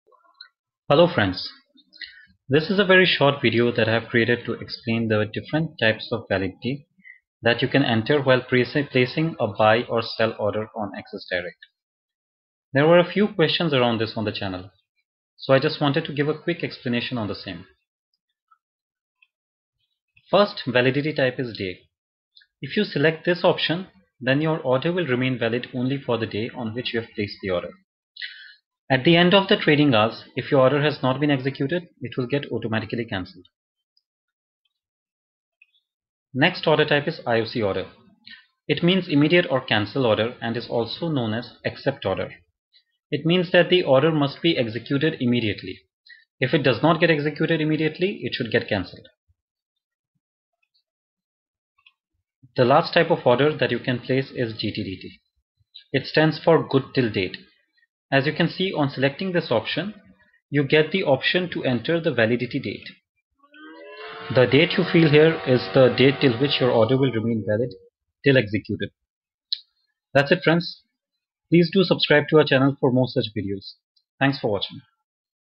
Types of Order Validity in Share Trading - Axis Direct
This is a short video to explain the different types of order validity that you can select while placing a buy or sell order on Axis direct.

This video is meant to answer the following questions -
What is day validity in trading?
What is IOC validity in trading?
What is GTDt validity in trading?

Video Created by S. N. Alam.